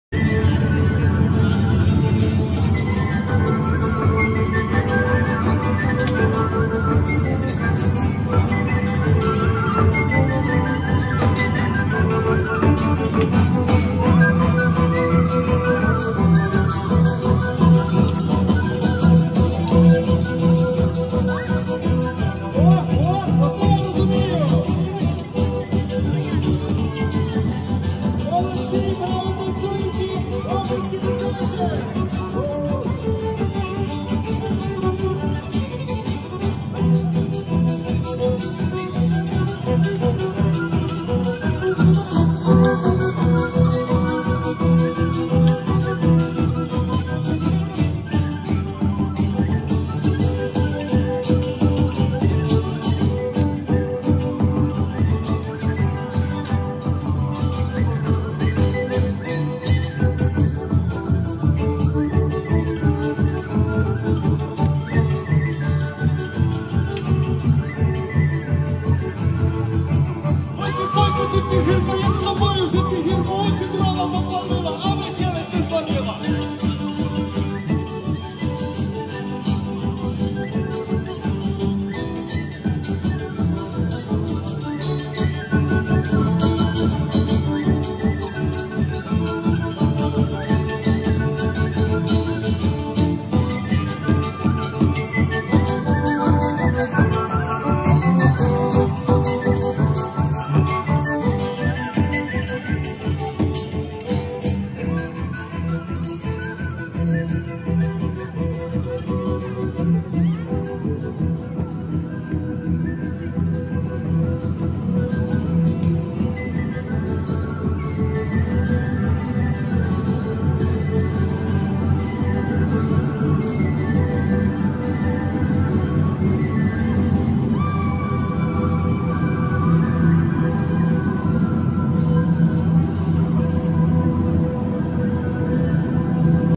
Ukraine War: Kyiv train station 19Dec22 after ruzzian attacks on the city... And the people danced!!
This is so inspiring and telling of the spirit of the Ukrainian determination and unbreakable spirit. No dictator, no country, no ideology can defeat this. Ukraine is unbreakable and will defeat putin and ruzzia and push them back to their shithole. Slava Ukrayini